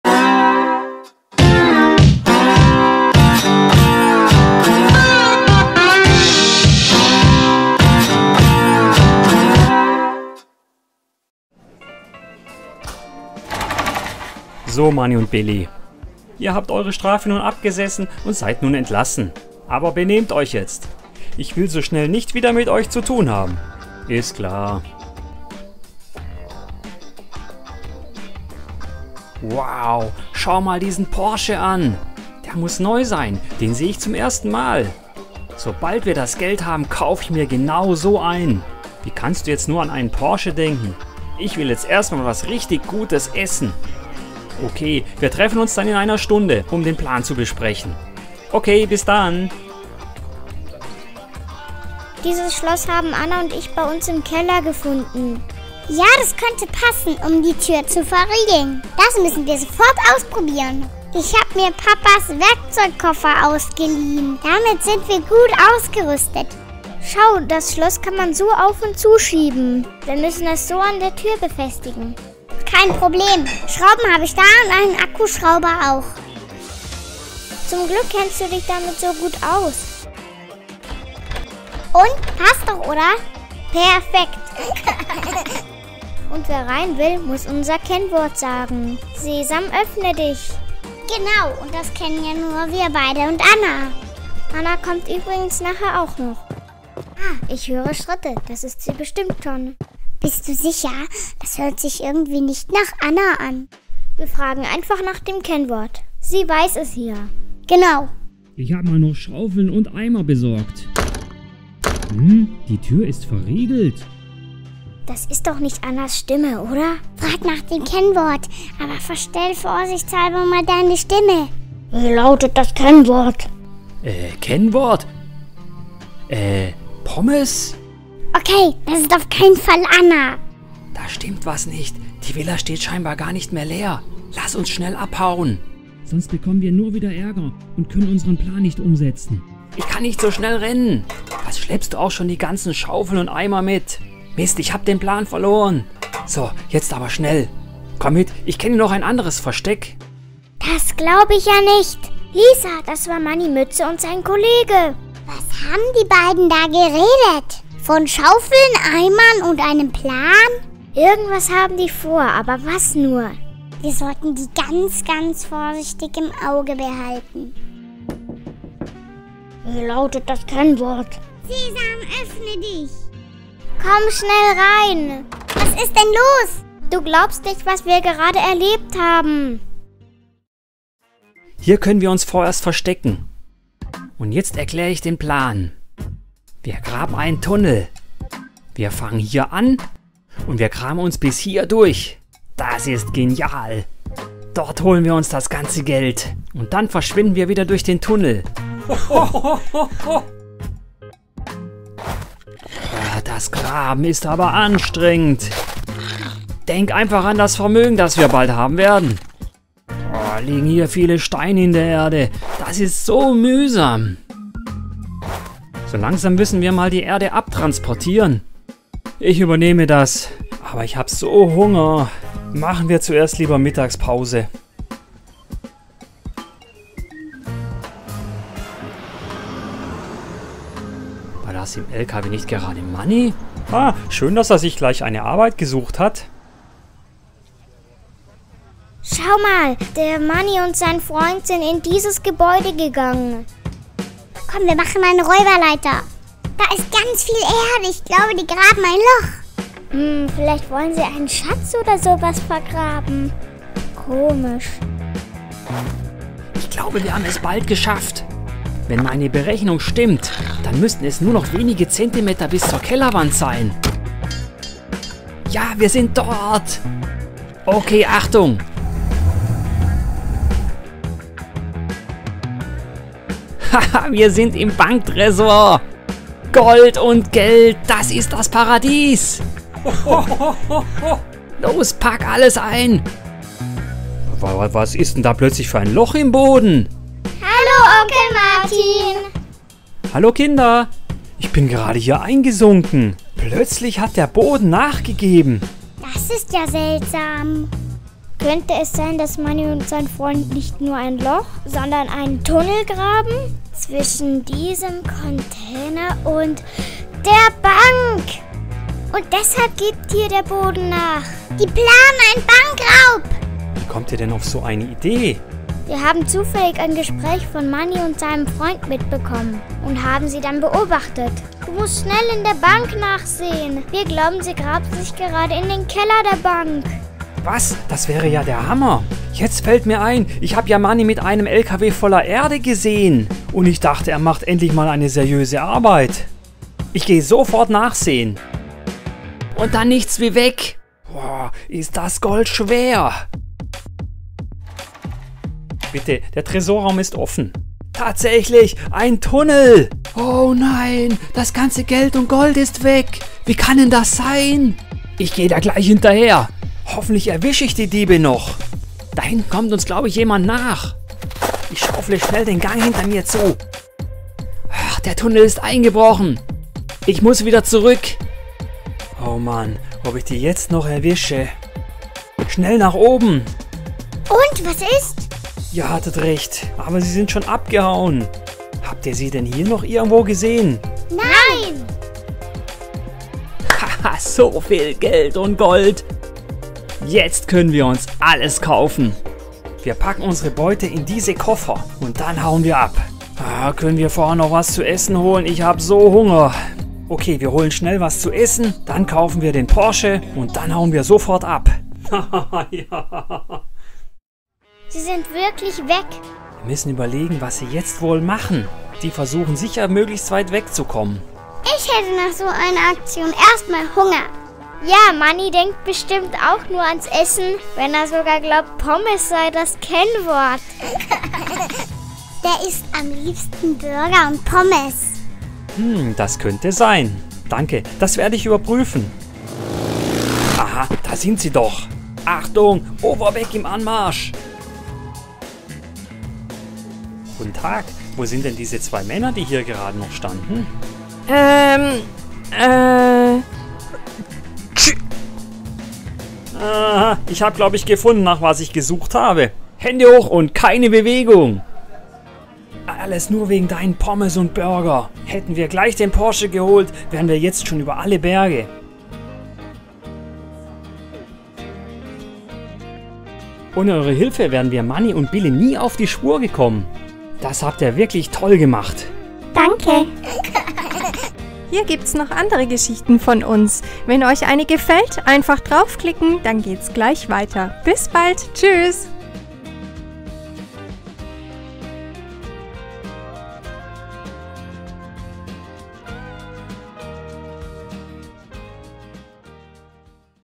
Playmobil Film Familie Hauser - Der große Raub - Kommissar Overbeck Polizei Geschichte für Kinder
PLAYMOBIL Polizei Kommissar Overbeck und Anna, Lena und Lisa von der gelben Villa ermitteln heute gemeinsam, um einen großen Raub aufzuklären. Viel Spaß, eure PLAYMOBIL Familie Hauser!

Auf unserem Kinderkanal Familie Hauser seht ihr tolle Kinderfilme.